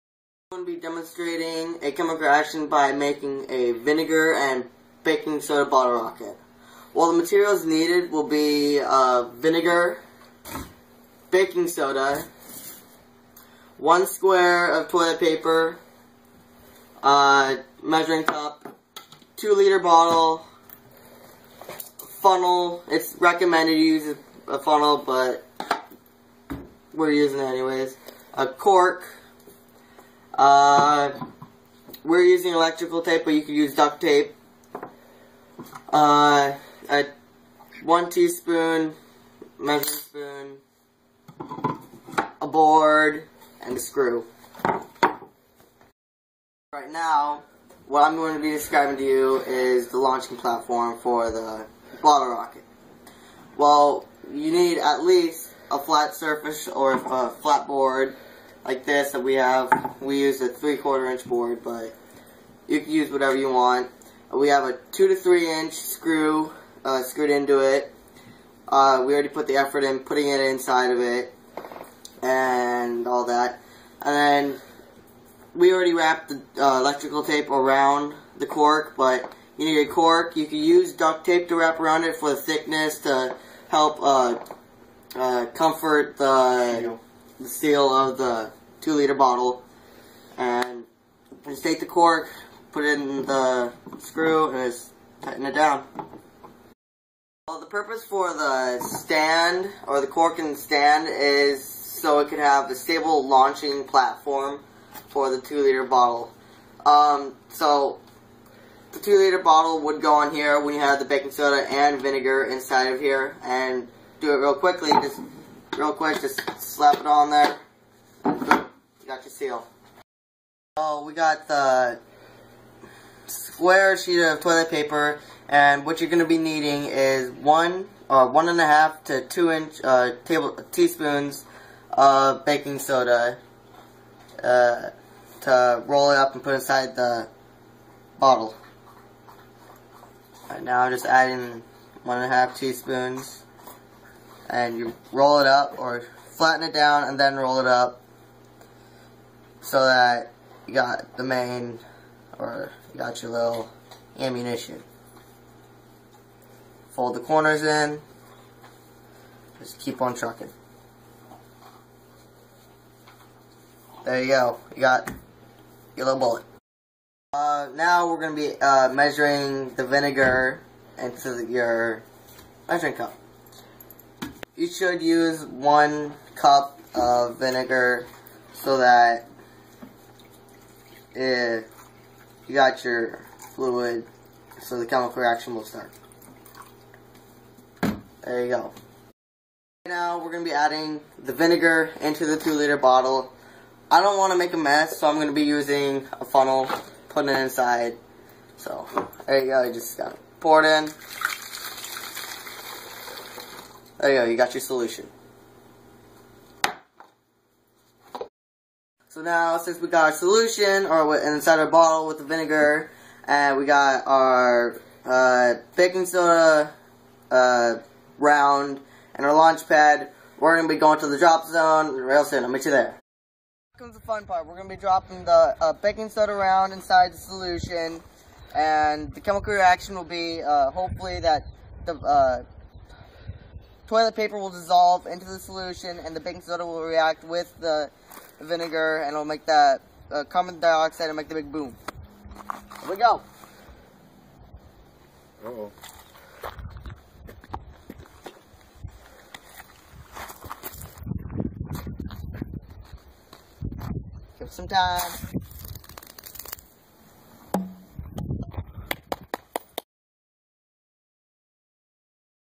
Chemical Reaction - Vinegar + Baking Soda fuels Bottle Rocket
Demonstration of the chemical reaction caused when baking soda (sodium bicarbonate) plus vinegar (acetic acid) yields carbon dioxide bubbles inside plastic bottle as rocket fuel.